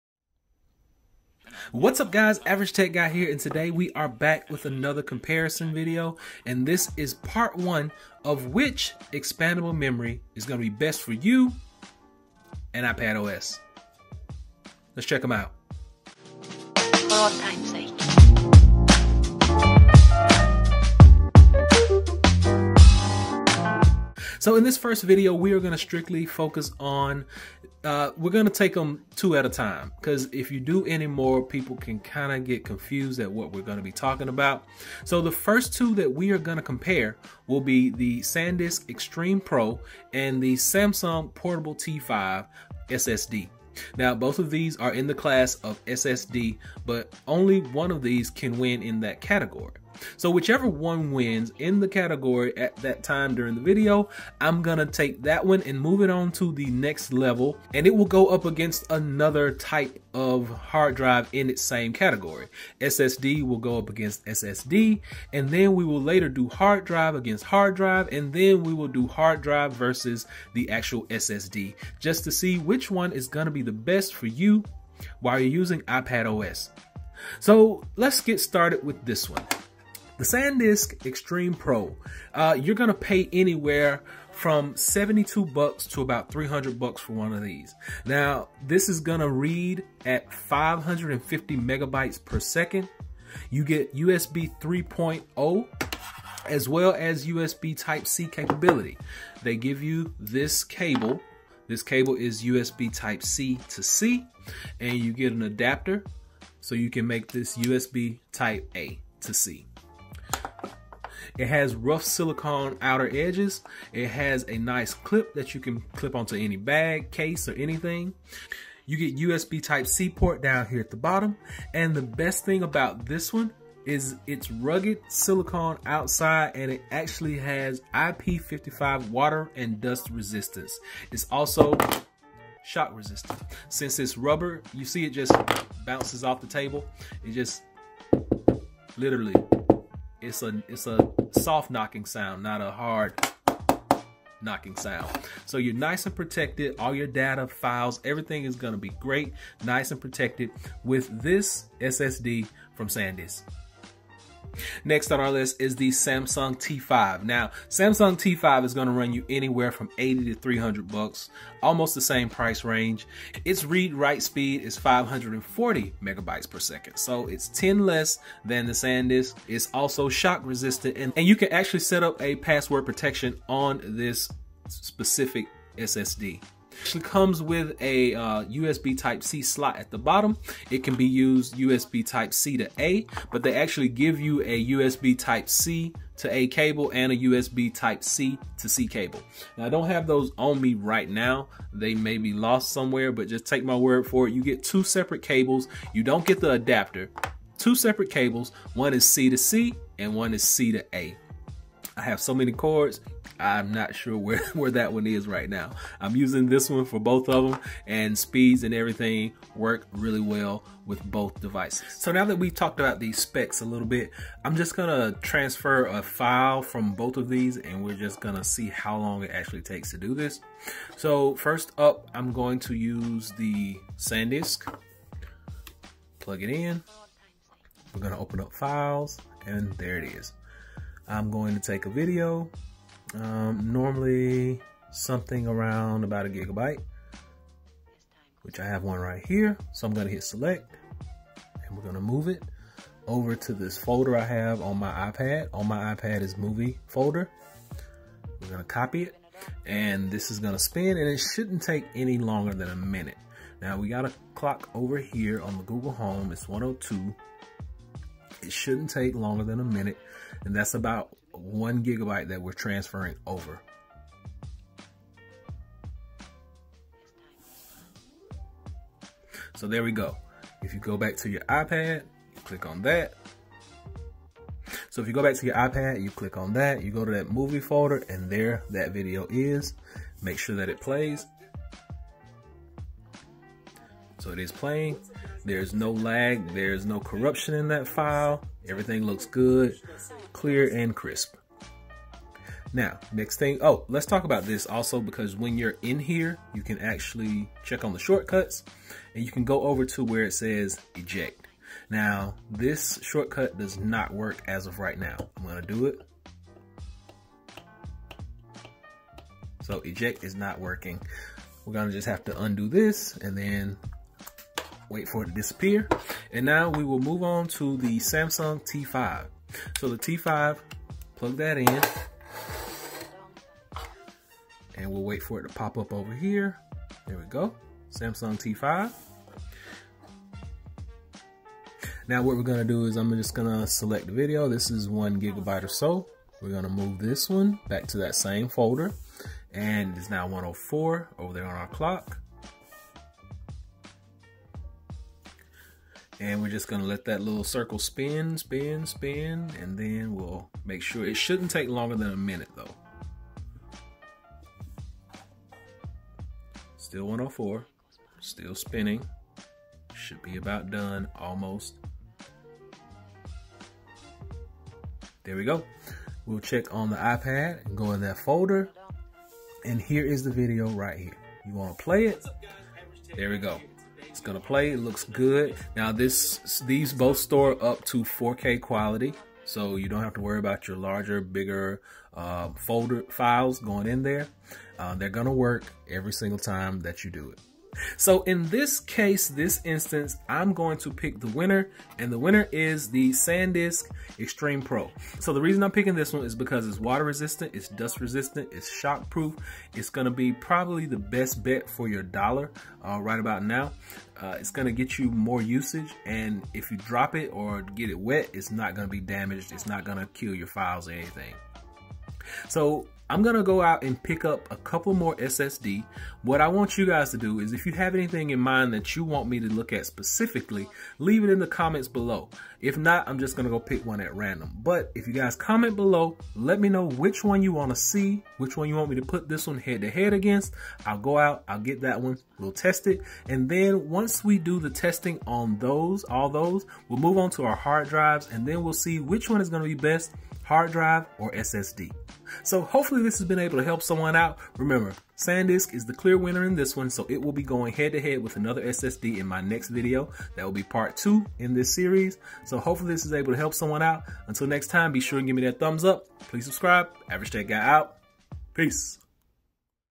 iPad Pro 11 iPadOs: Best SSD for Your Money...[Part 1]
Which Memory type goes best with iPadOs???
hdd, ssd, or something else???